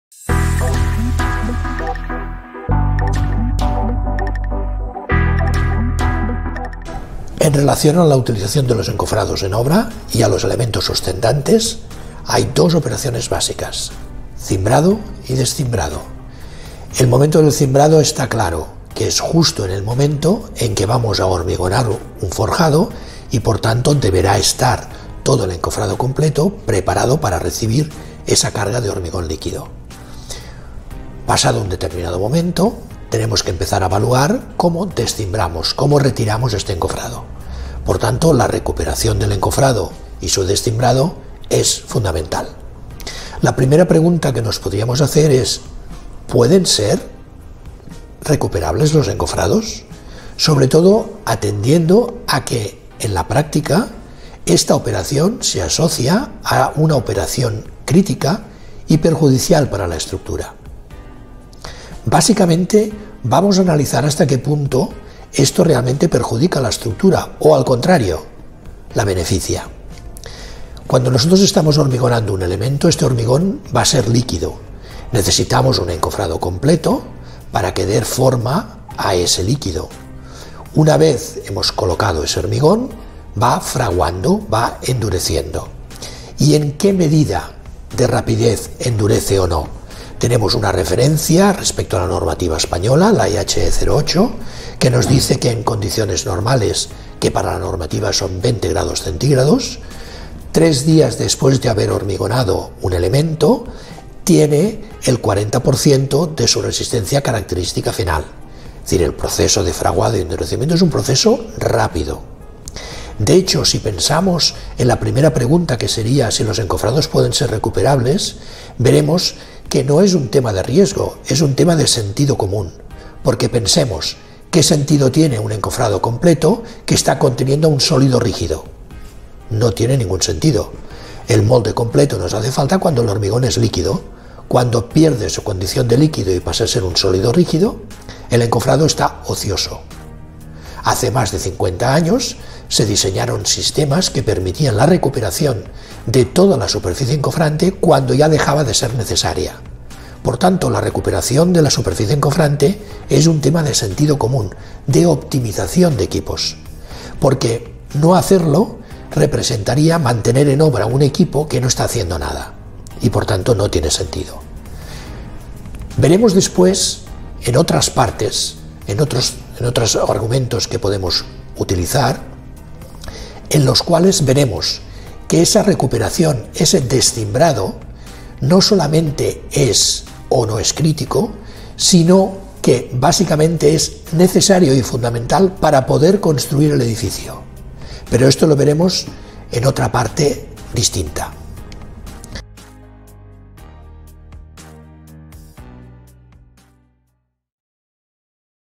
📣 Video Forum Alsina | Shoring Calculation: Recovery of Formwork
In relation to the use of formwork on site and the supporting elements, there are two basic operations. Scaffolded and unscaffolded. In this video we explain each of these concepts.